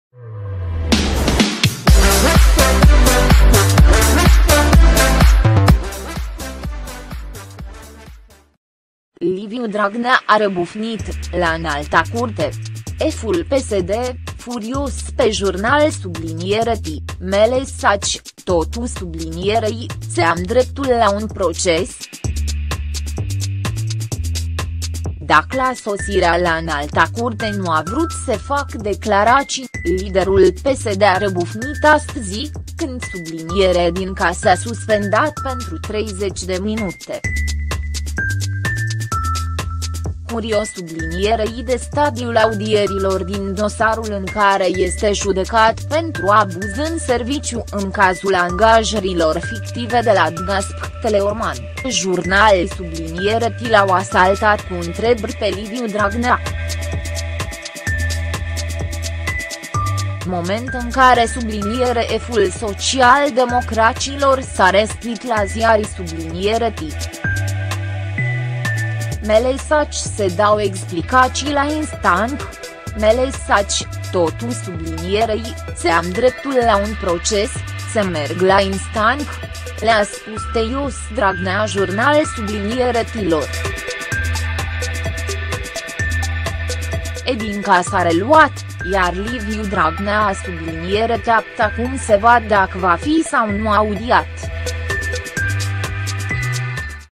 Liviu Dragnea A RĂBUFNIT, la Înalta Curte! Şeful PSD, FURIOS pe jurnalişti: 'Mă lăsaţi, totuş
Liviu Dragnea A RĂBUFNIT, la Înalta Curte! Şeful PSD, FURIOS pe jurnalişti: 'Mă lăsaţi, totuşi, să am dreptul la un proces?'